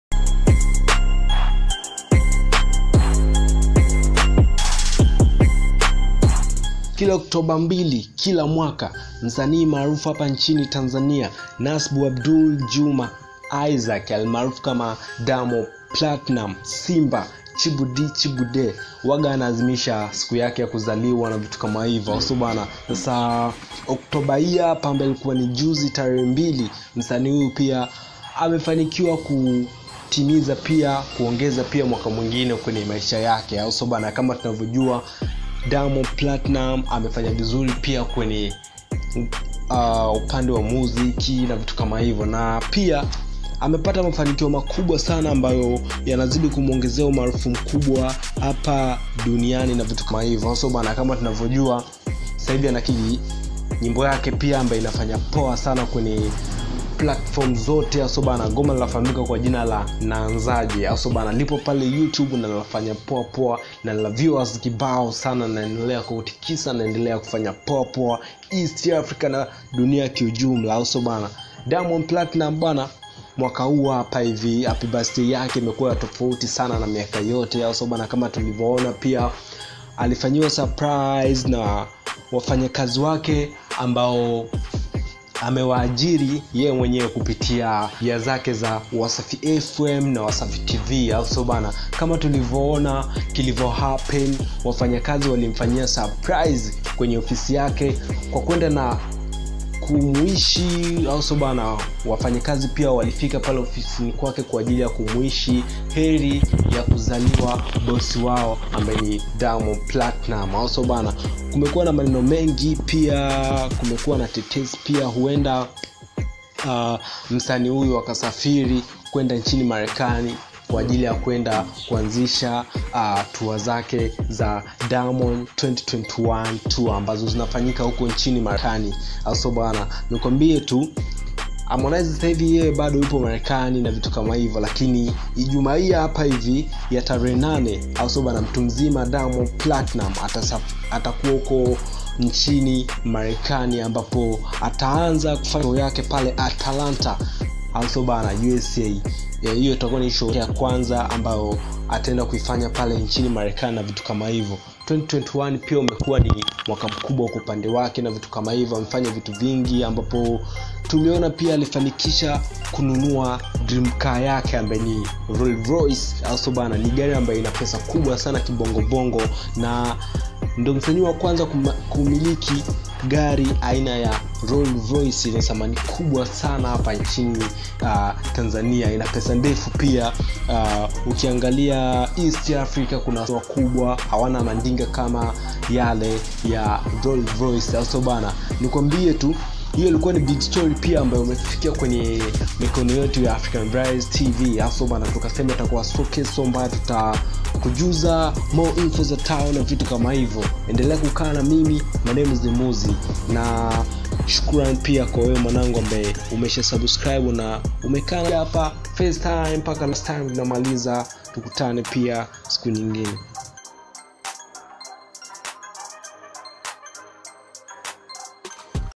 DIAMOND PLATINUM | HARMONIZE HAPPY BIRTHDAY YAKE | ASHAMBULIWA NA WAFANYAKAZI WAKE | MAREKANI#shorts
kila October 2 msanii diamond platnum huadhimisha siku yake ya kuzaliwa na mwaka huu pia alifanikiwa kuongeza mwaka mwingine kwenye maisha yake